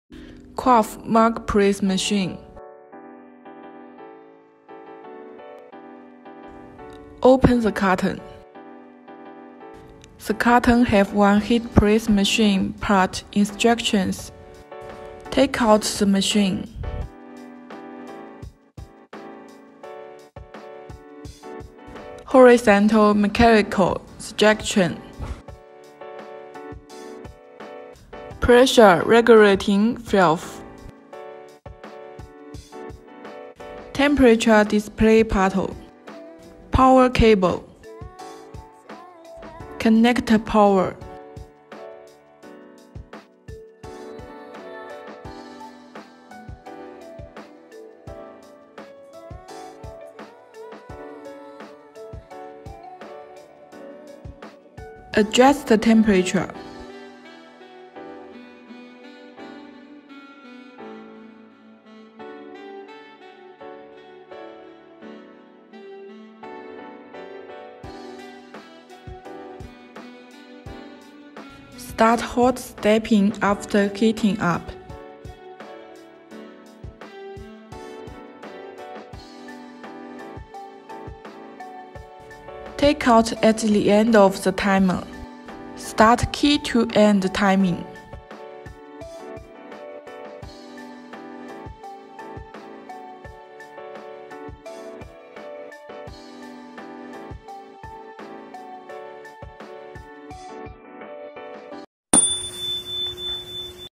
QUAFF Unpacking Video of Grip Cup Copying Machine
Machine structure, temperature setting, hot stamping process